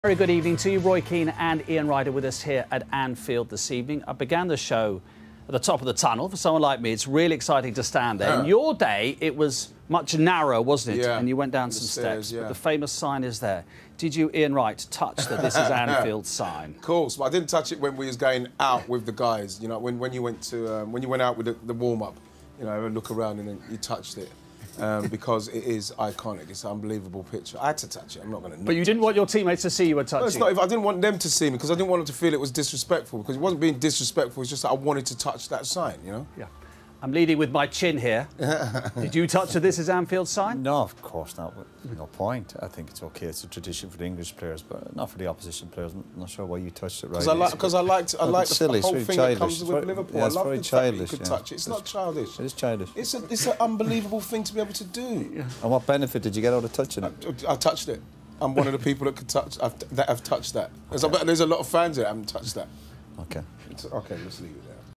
"It's NOT childish!" - Roy Keane & Ian Wright have it out over the Anfield sign 😆
Roy and Ian have a HEATED... disagreement over the 'this is Anfield' sign at Liverpool FC.